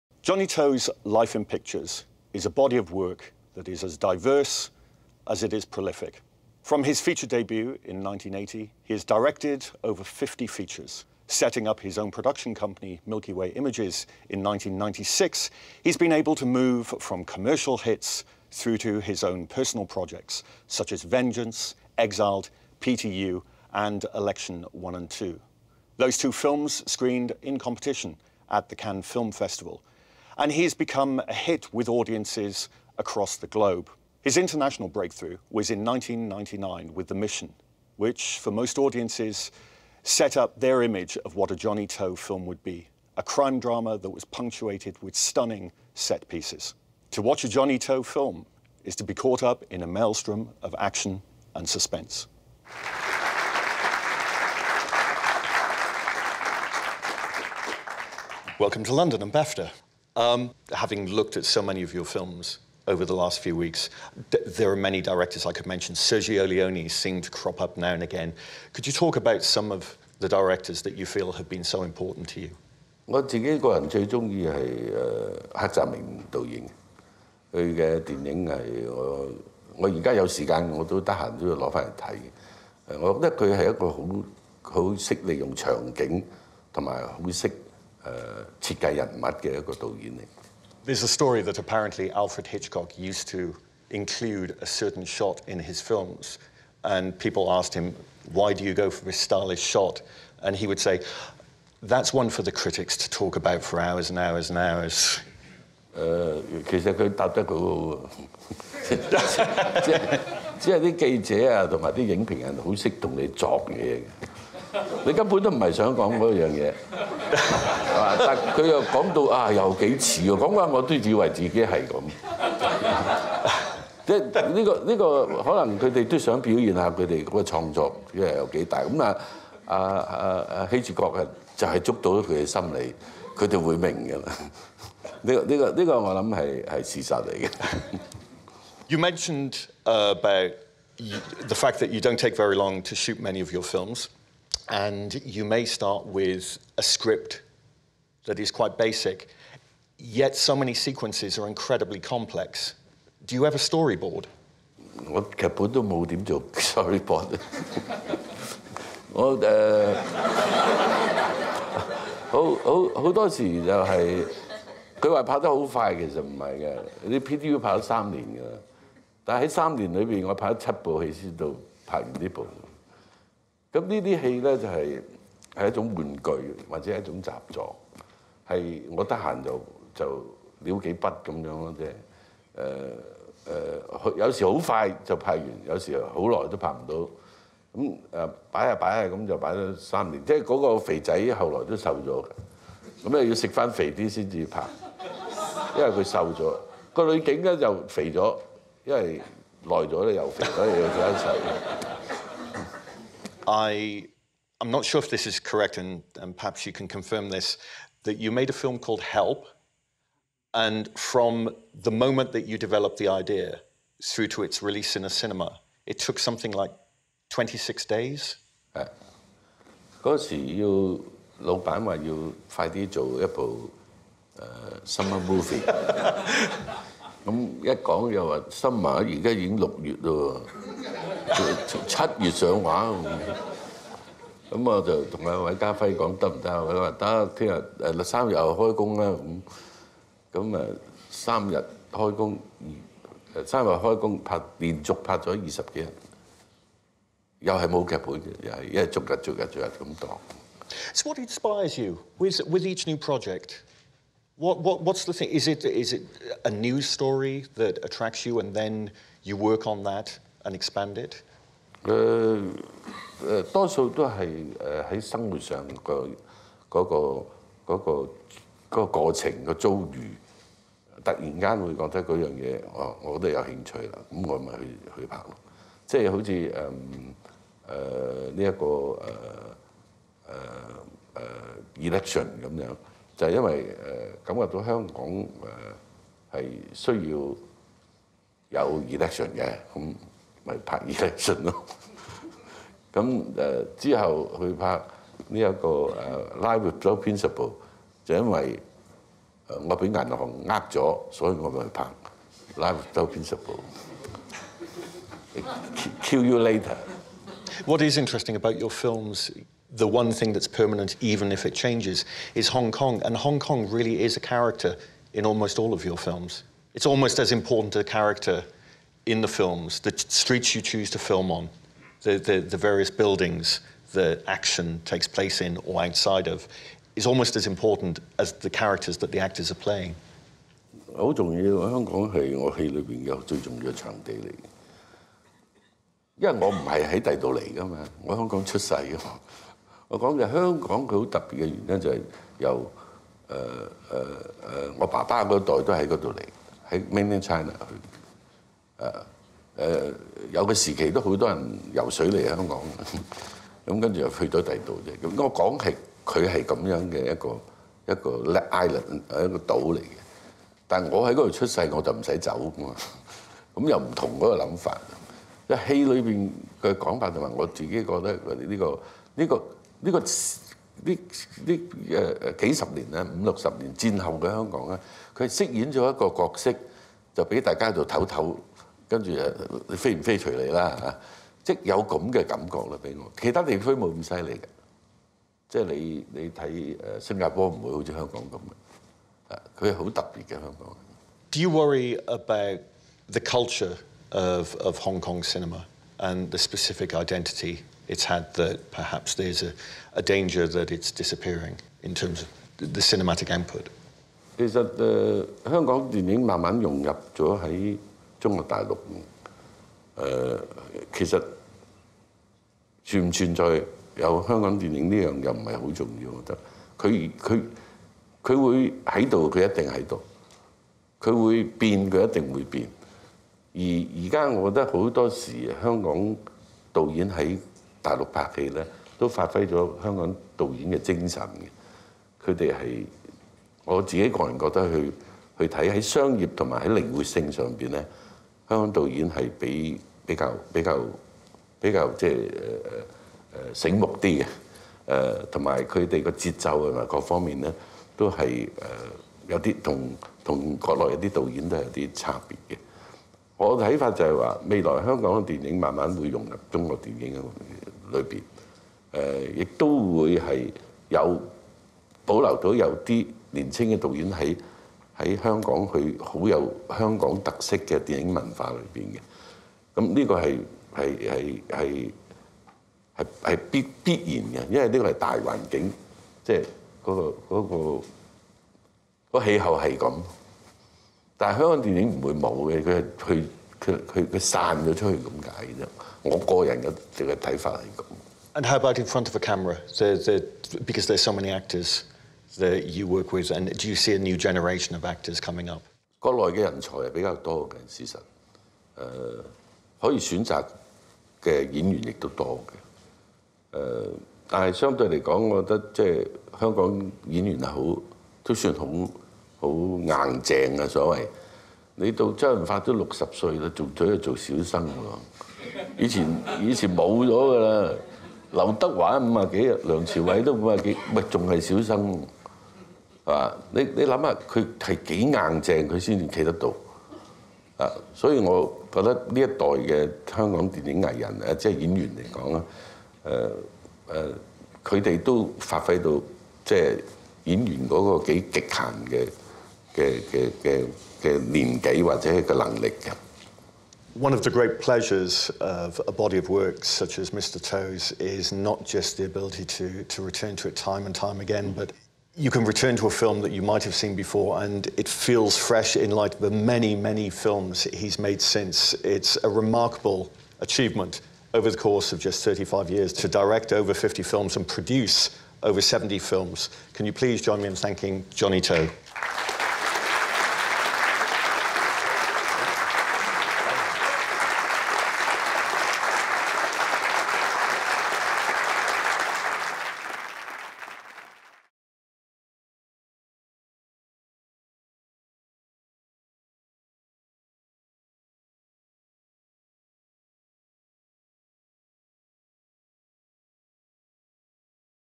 Masterclass Series: A Life in Pictures: Johnnie To at BAFTA | 大師班系列：BAFTA光影人生：杜琪峯
Hong Kong director and producer Johnnie TO looked back on his wide-ranging career in a fascinating BAFTA A Life in Pictures on-stage interview with Ian Haydn Smith, Curzon Magazine editor, co-presented by the Asian Film Awards Academy and the British Academy of Film and Television Arts (BAFTA). The legendary Hong Kong filmmaker discussed working without a script, drawing inspiration from everyday life and the power of Hong Kong as a character in his films.